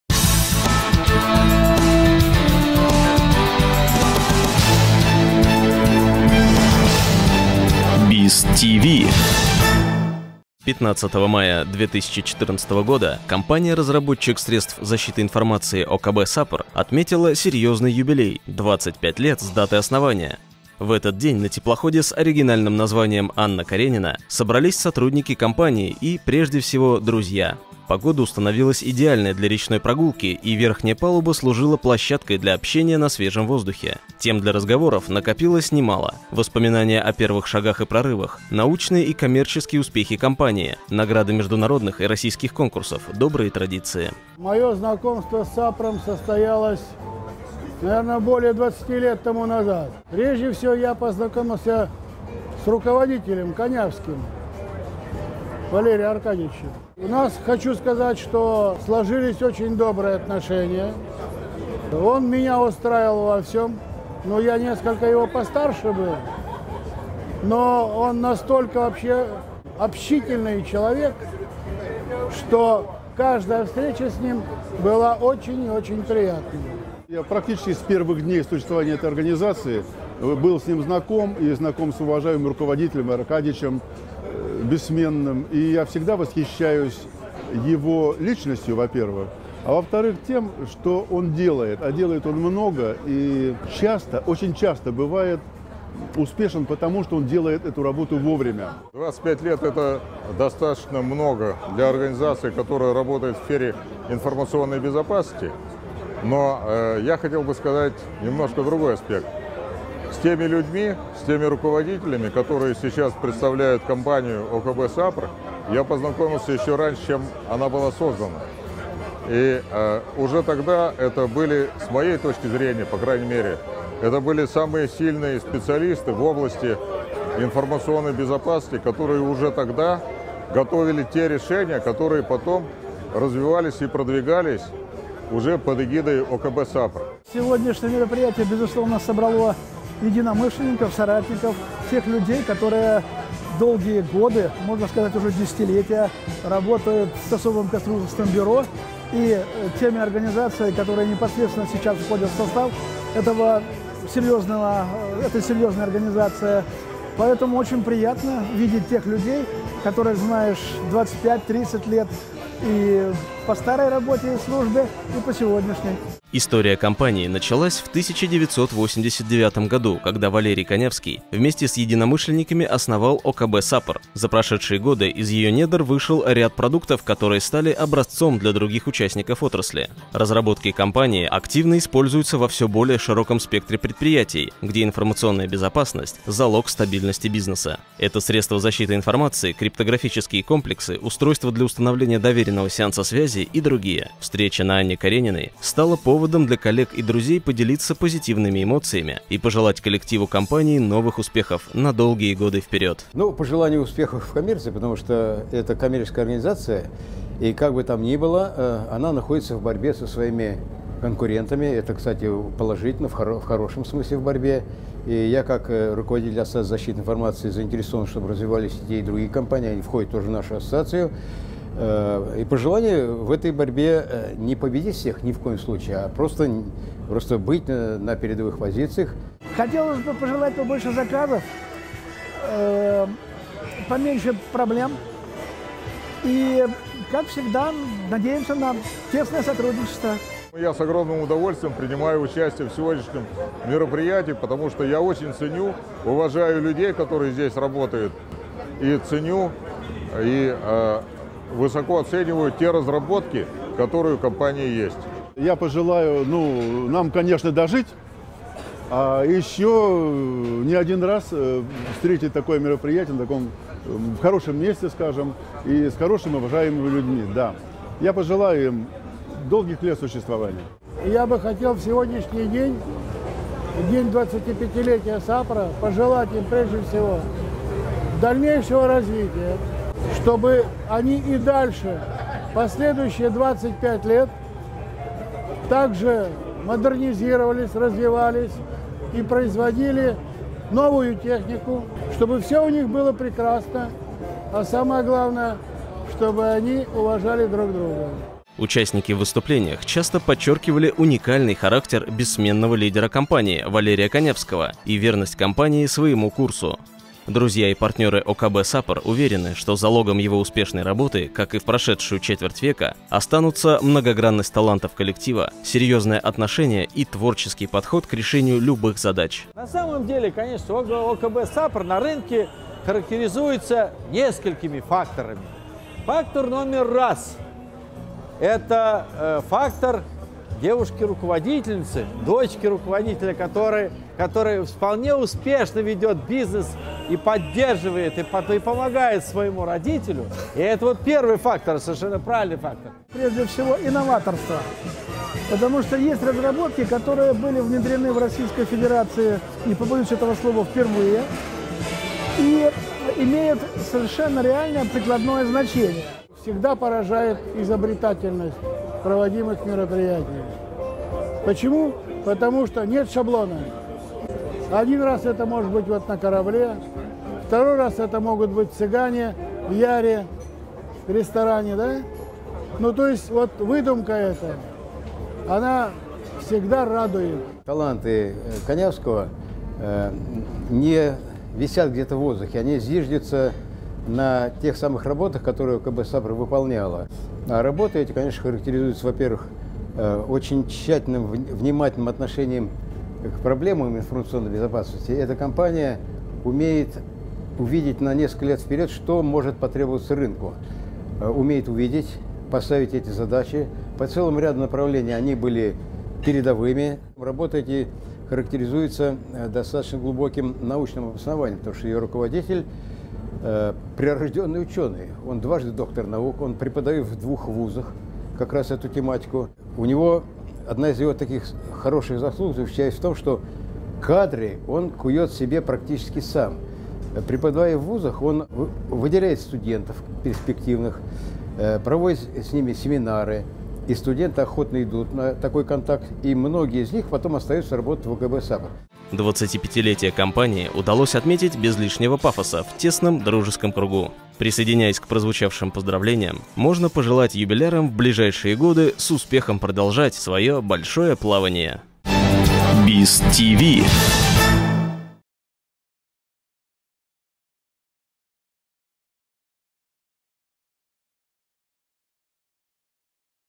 BIS TV - Выпуск №32: Четверть века ОКБ САПР
15 мая 2014 года компания-разработчик средств защиты информации ОКБ САПР отметила серьёзный юбилей — 25 лет с даты основания. В этот день на теплоходе с оригинальным названием «Анна Каренина» собрались сотрудники компании и, прежде всего, друзья.

Вспоминая прошедшую четверть века, участники встречи делятся воспоминаниями о первых встречах, закономерных успехах и восхищаются талантами коллектива ОКБ САПР. Воспоминаниями и пожеланиями делятся: А. Каландин, В. Пярин, А. Курило, Г. Емельянов, И. Гвоздев, А. Баранов.